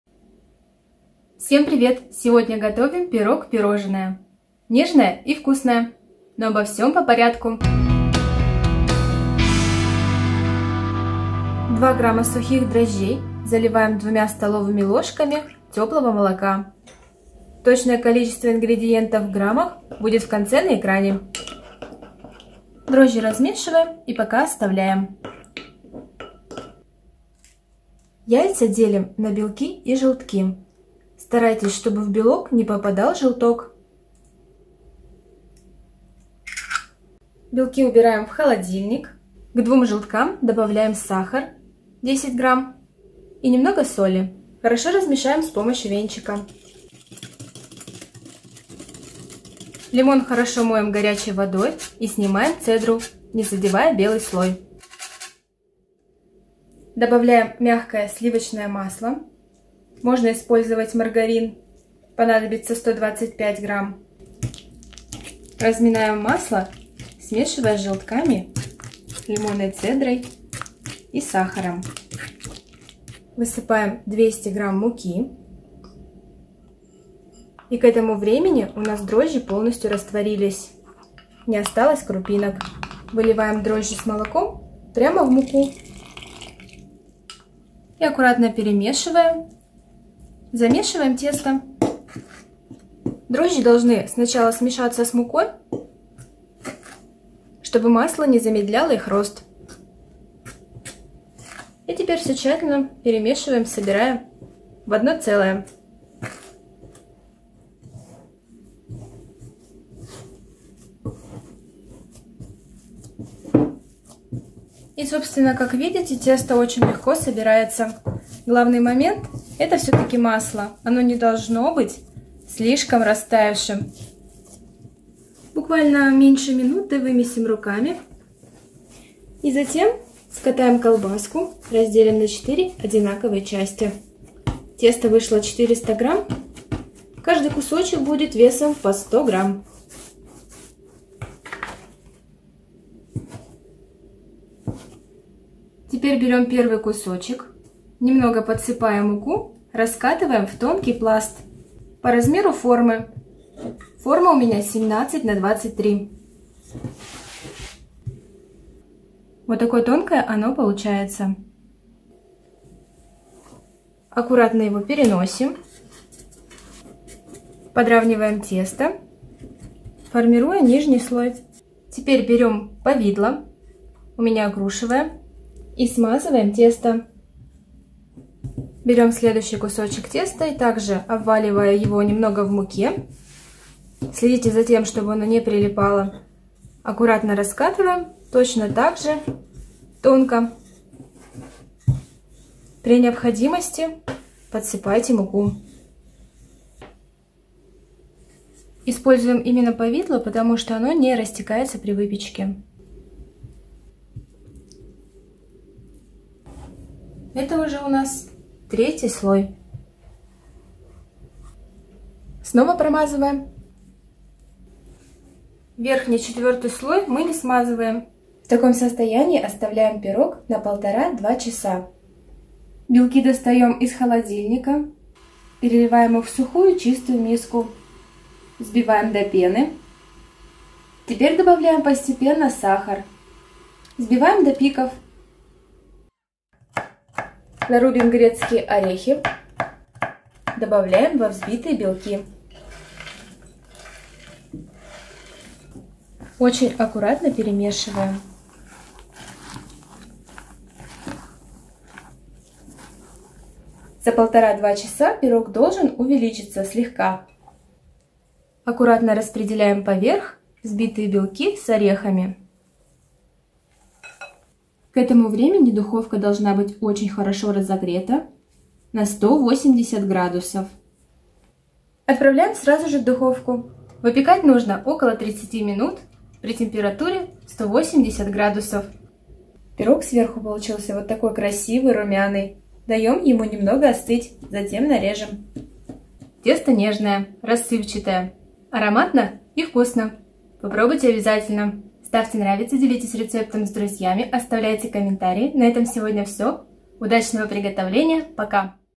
Просто раскатываем и смазываем повидлом. НОВОЕ Пирожное к чаю! Будто СЛОЕНОЕ. Мы в восторге!
Пирог по этому рецепту получается изумительно вкусным! Нарежем ☝️ кусочками и получим скорее слоеное пирожное или даже торт. Рецепт абсолютно новый, готовила по нему впервые и результат нашу семью впечатлил. Советую однозначно! Здесь тесто прослаивается повидлом, так как оно не течет при высокой температуре, а сверху безе с орехами. Безе скорее ❗ жевательное, не сухое, так как при взбивании белков добавляется мало сахара, если хотите сухое, то и сахара нужно больше (Хотя, куда больше?) Пирог и так сладкий! Тесто песочное на дрожжах, поэтому и времени на расстойку уходит много, но результат этого стоит: рассыпчатое, мягкое и такое нежное… ммм… попробуйте! Ингредиенты в ложках и стаканах найдете ниже. Удачной выпечки!

👉 Оставьте комментарий и жмите 👍 "Нравится" - это лучшая благодарность от Вас!

✅Ингредиенты для приготовления слоеного сладкого пирога с безе и орехами, а также пошаговый рецепт с фото-описанием

0:01 Всем привет!
0:12 Опара
0:39 Тесто
2:24 Сборка пирога
2:34 Размер формы
3:43 Безе (для сухого безе нужно больше сахара)
4:34 Температура и время выпечки
4:46 В разрезе
4:52  Количество ингредиентов в точных граммах

✅ Ингредиенты:
👉Тесто:
дрожжи сухие – 2 г = 0,5 ч.л. без горки
молоко – 30 мл = 2 ст.л.
желток – 2 шт.
сахар – 10 г = 2 ч.л.
соль – 1 г = пара щепоток
цедра половины лимона
сливочное масло – 125 г = 0,5 стакана
мука  – 200 (+/-)  г = 1 стакан + 2 ст.л.
👉Дополнительно:
белок – 2 шт.
орехи – 50 г = небольшая горсть
повидло – 200 г = 3 ст.л. с горкой

❗Форма 17х23 (можно просто на большой противень)
*стакан 250 мл